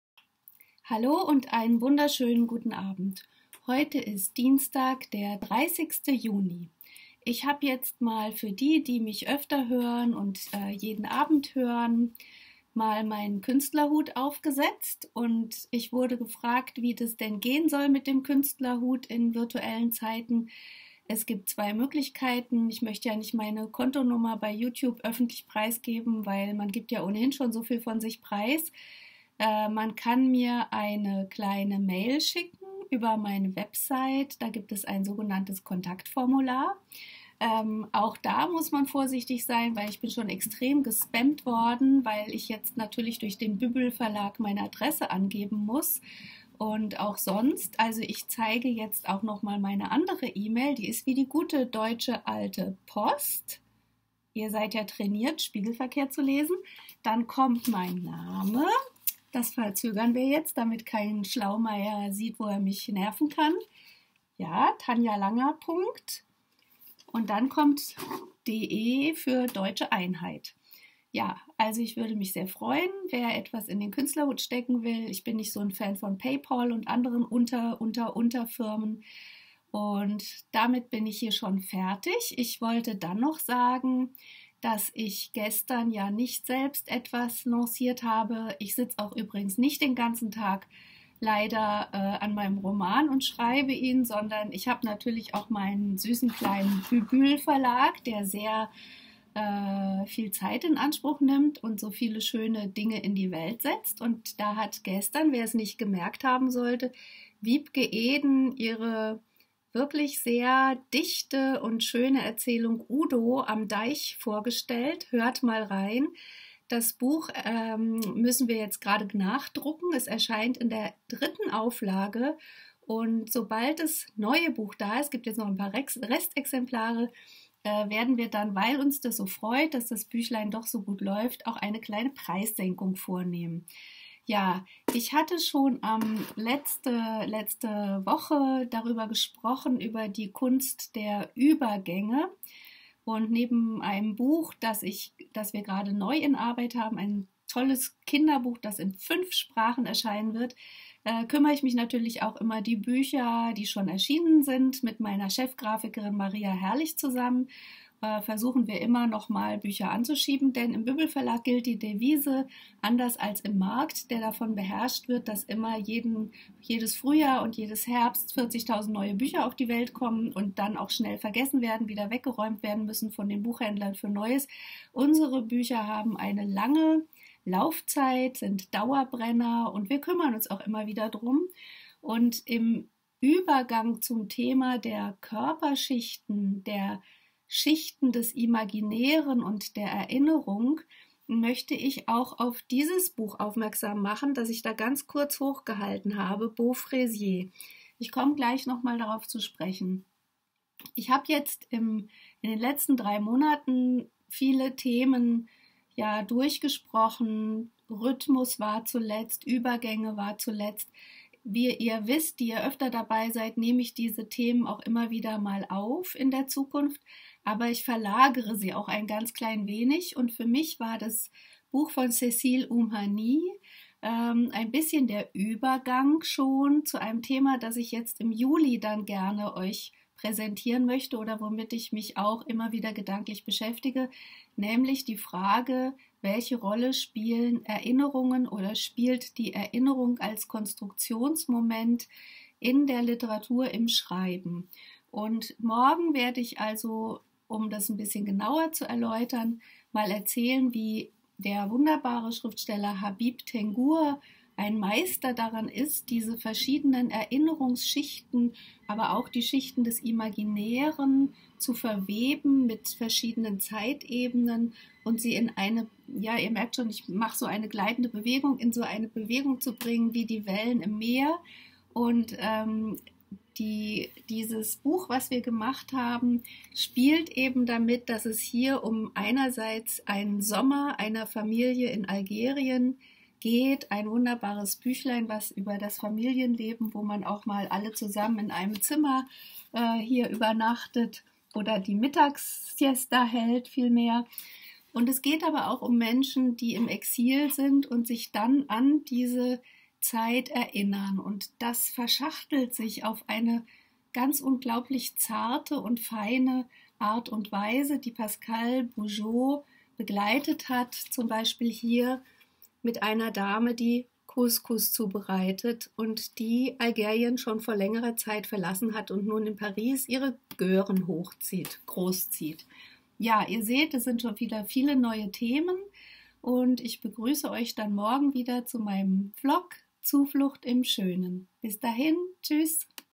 Zuflucht im Schönen No. 68: Übergang zu einem neuen Thema - Erinnerung von Tanja Langer
Drei Monate lang habe ich über verschiedene Themen gesprochen, die ich auch immer wieder aufgreife, wie zuletzt den Rhythmus oder die Kunst der Übergänge -
Cécile Oumhanis Roman bot  einen eleganten Übergang zu einem neuen Thema, das im Juli Schwerpunkt sein soll (neben meinen üblichen Reaktionen auf diese Welt): Schichten der Erinnerung und Erinnerung als Konstruktionsprinzip in der Literatur.

Als Beispiel möchte ich über den algerisch-französischen Dichter Habib Tengour, der auf meisterhafte und schwebende Weise Erinnerung und Imagination in Schwingung bringt: im Buch "Beau Fraisier - Mein schönes Erdbeerfeld", das Pascale Bougeault mit ihren Zeichnungen begleitet und Regina Keil-Sagawe übersetzt hat,
zu finden im Bübül Verlag Berlin!

Aus meinem Bübül Verlag - der sich nicht dem Rhythmus des Marktes unterwirft - stellte gestern hier auf meinem Kanal auch die Schriftstellerin Wiebke Eden ihre wunderbare Erzählung "Udo" vor:

und: bestellt!

Zum Thema Künstlerhut: Bitte eine Mail über die website schicken!